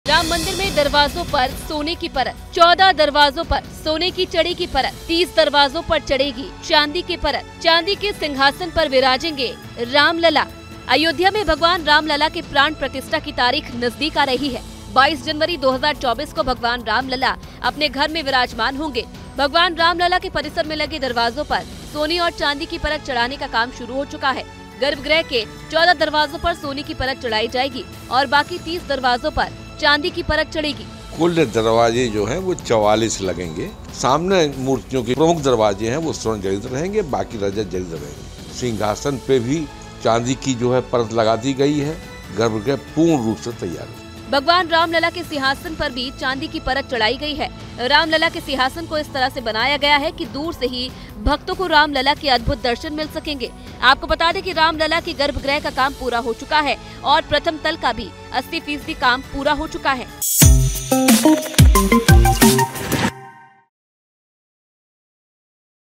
Ayodhya: Ram Mandir में सोने-चांदी के कुल कितने होंगे द्वार?, कैसा होगा रामलला का सिंहासन?ABP LIVE
News: भगवान राम लला के गर्भ गृह में लगे दरवाजों पर सोने की परत चढ़ाने का काम शुरू हो चुका है.. राम मंदिर में सोने-चांदी के कुल कितने होंगे द्वार और कैसा है रामलला का सिंहासन आइए जानते हैं|